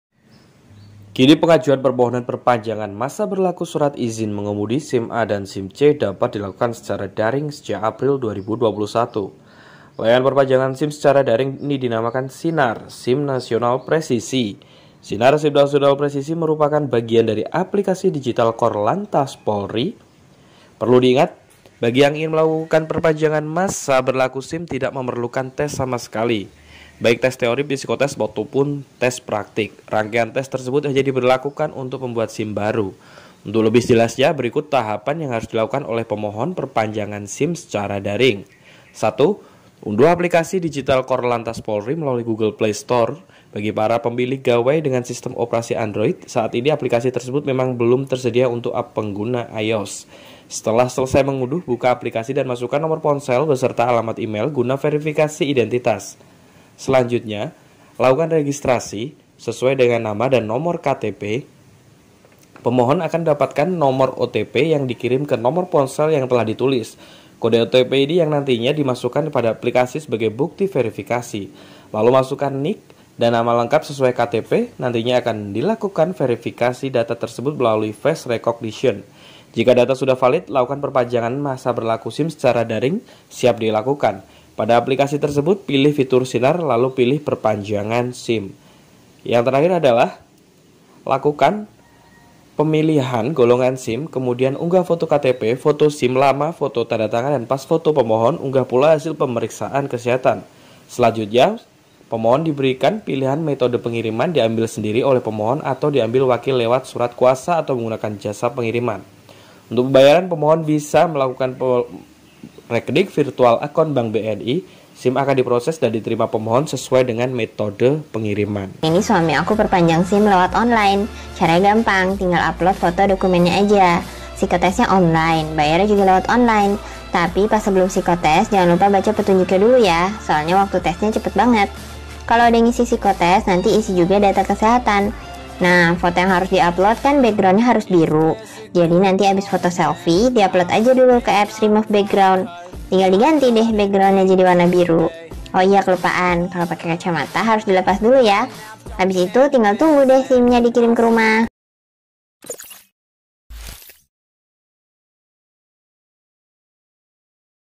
TAK ADA PUNGLI, INI CARA PERPANJANG SIM ONLINE, ADA UJIAN RESMINYA JUGA LHO! SIMAK TATA CARANYA!
Cara Perpanjangan SIM Online, Ini Daftar Biayanya

Kini pengajuan permohonan perpanjangan masa berlaku surat izin mengemudi (SIM) A dan C dapat dilakukan secara daring sejak April 2021.

Layanan perpanjangan SIM secara daring ini dinamakan SINAR (SIM Nasional Presisi).

[editor : aj]